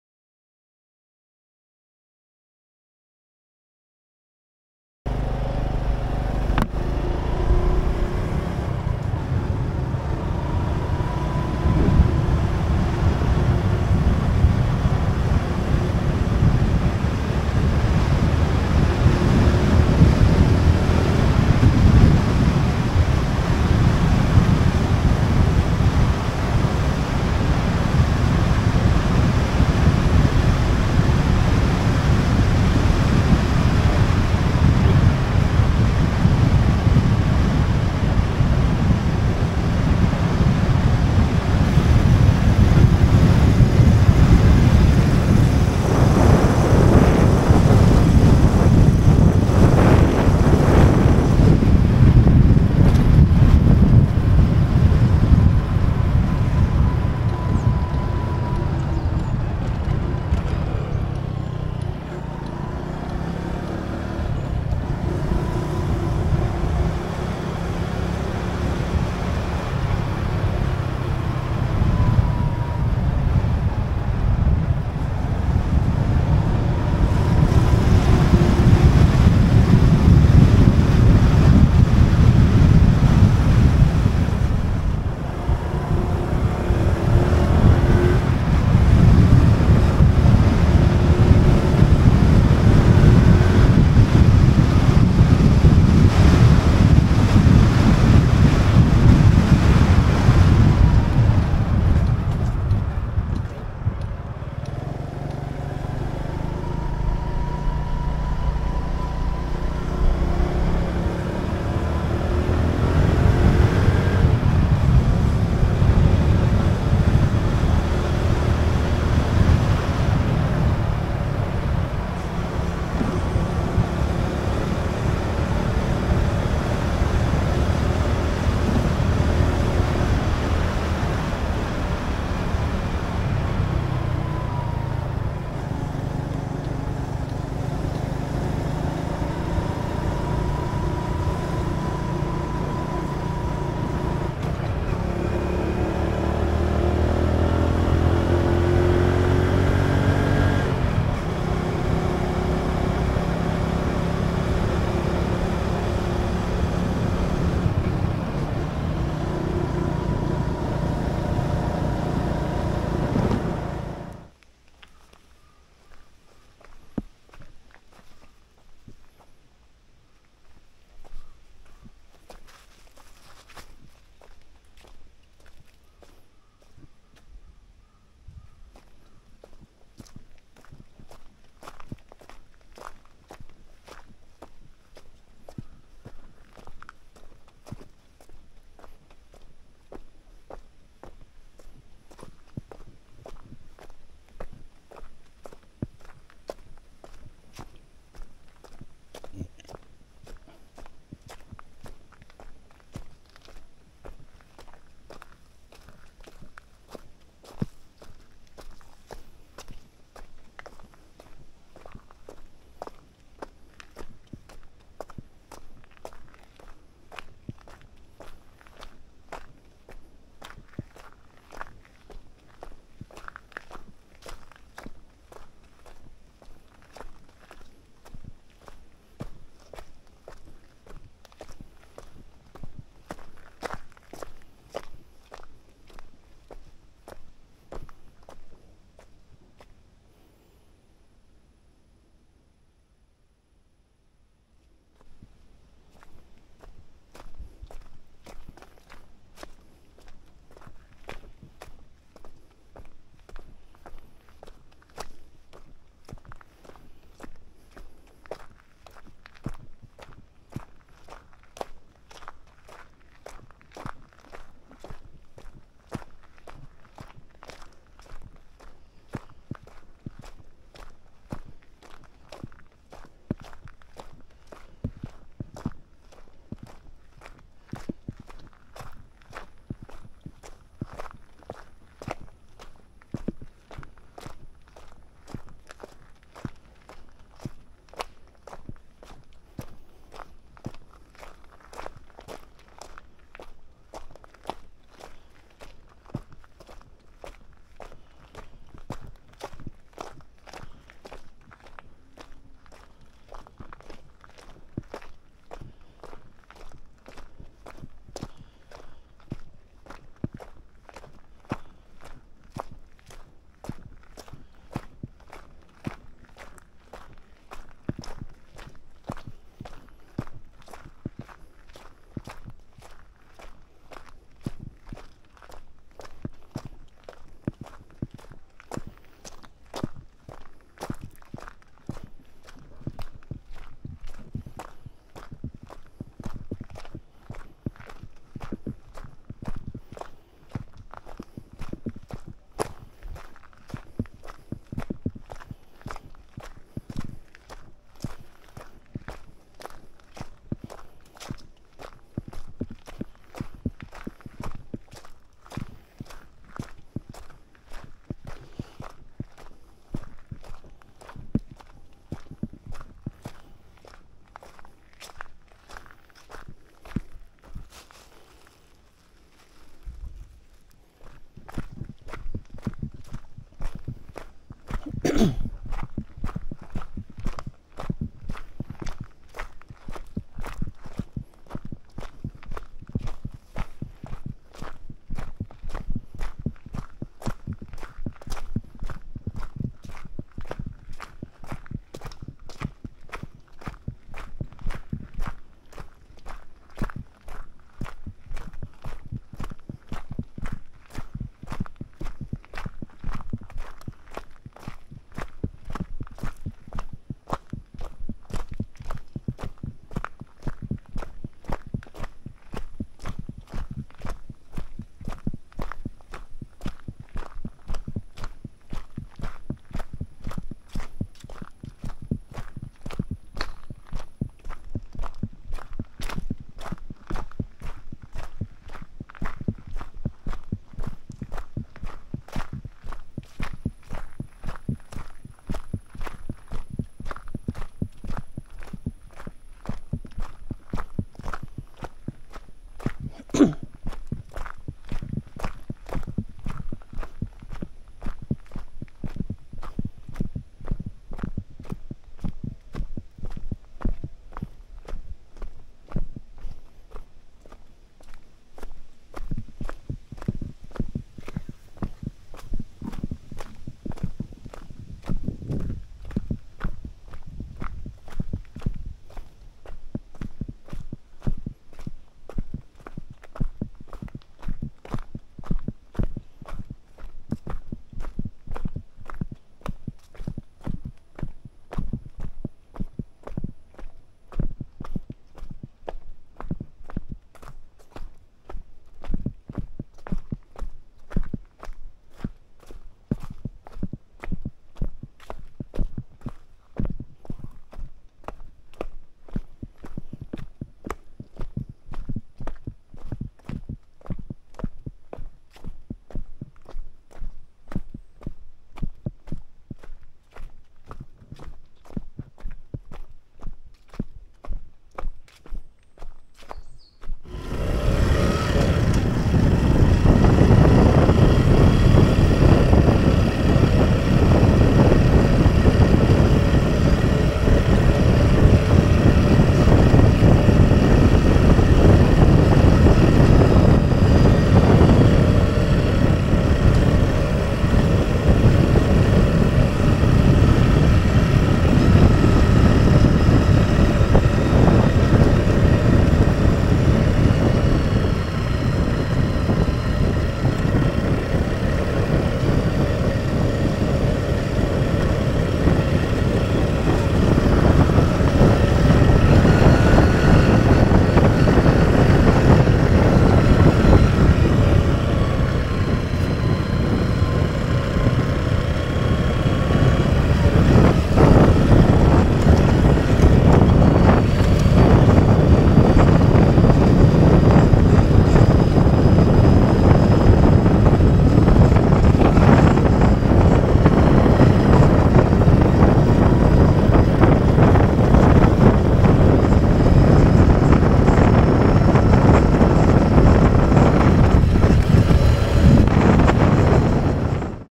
[4K 60fps] DJI OSMO Action 3 First touch. Better than GoPro?
New DJI action cam OSMO Action3 by walk and on Moto